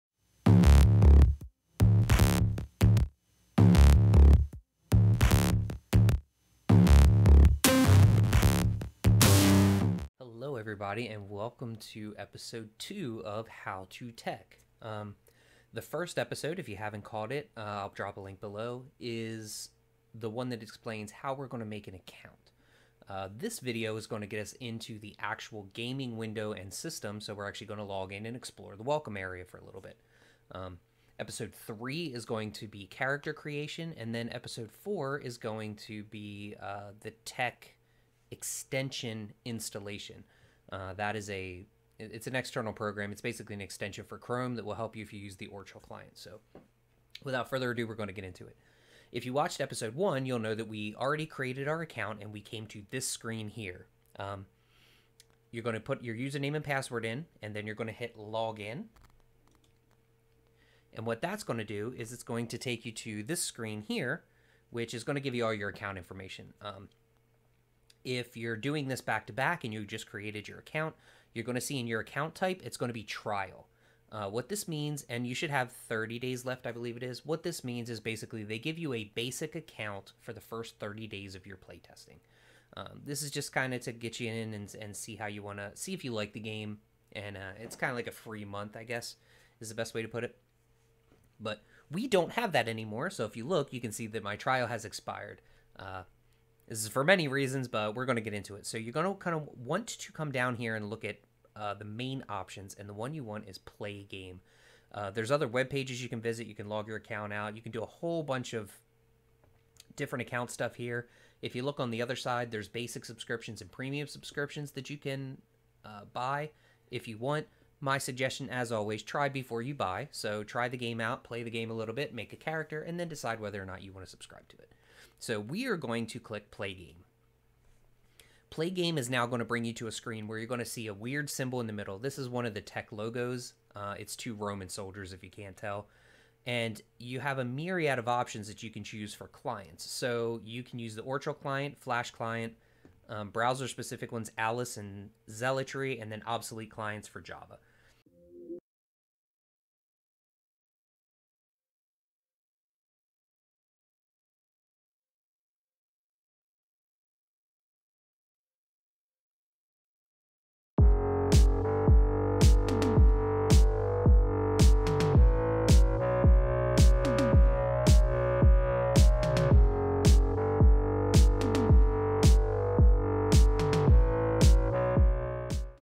Episode 2 - Welcome Area
This episode will teach you howe to access the game and log into the actual client to play. Unfortunately, there was an issue with the 2nd video clip for this, so I had to finish it with text on the screen.